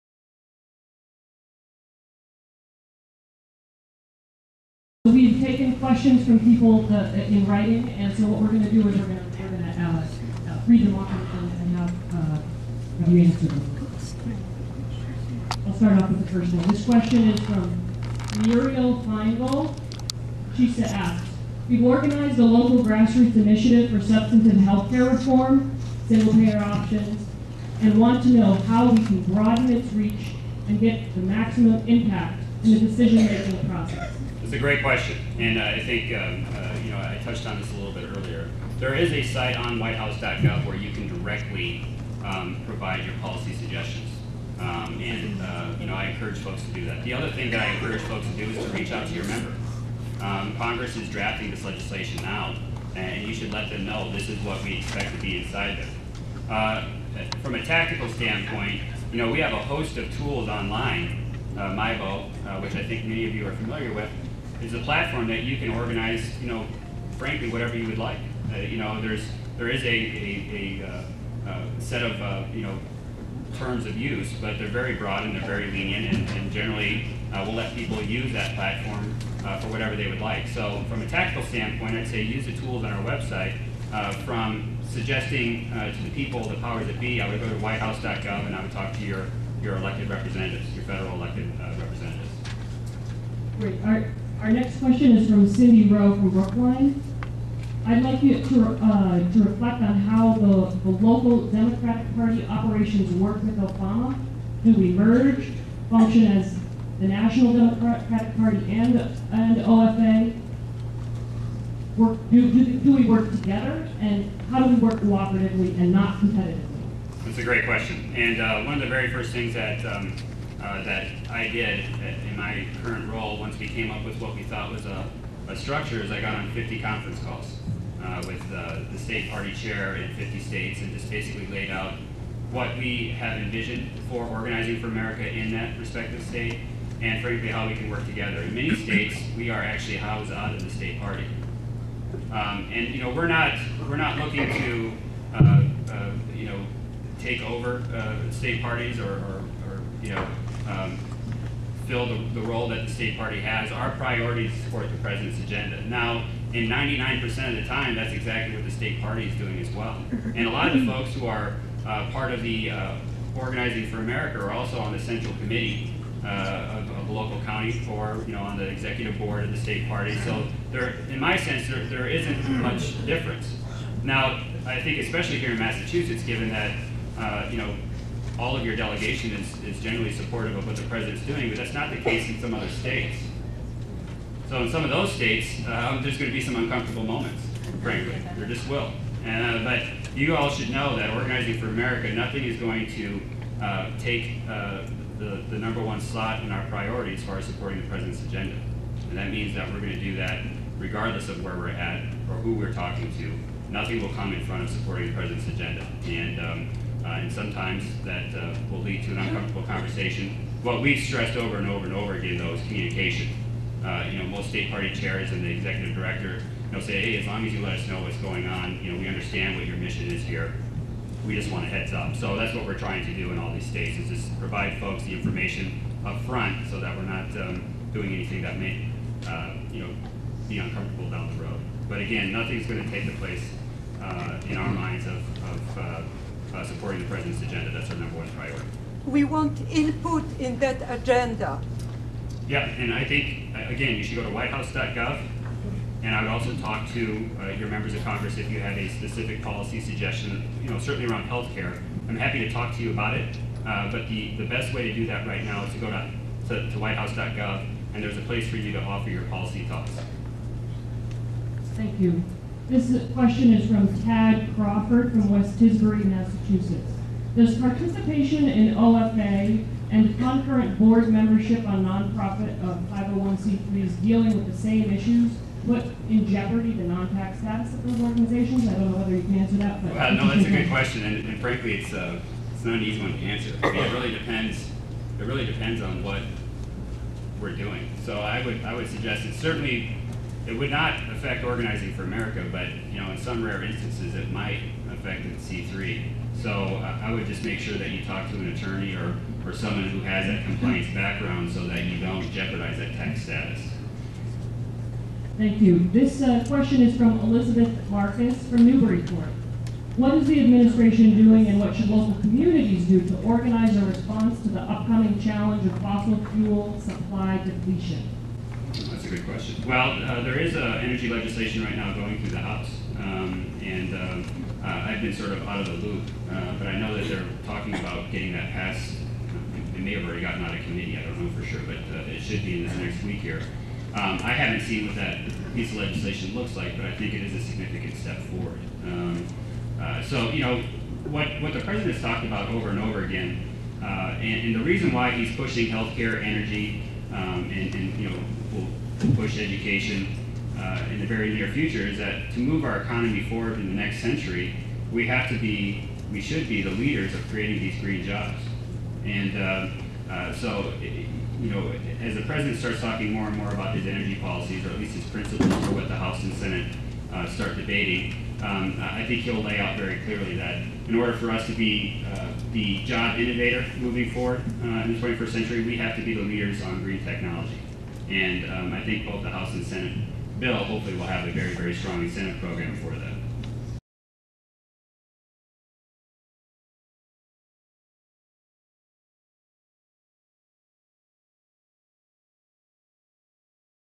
OFA-MA Grassroots Organizing Forum(Mitch Stewart )
Mitch Stewart answer questions from the grassroots .

How do you affect the decision making process?
What is OFA relationship to the Democratic Party?
Does belong to OFA conflict with belongs to issue based groups?
What is admonition doing about the fossil fuel depletion problem?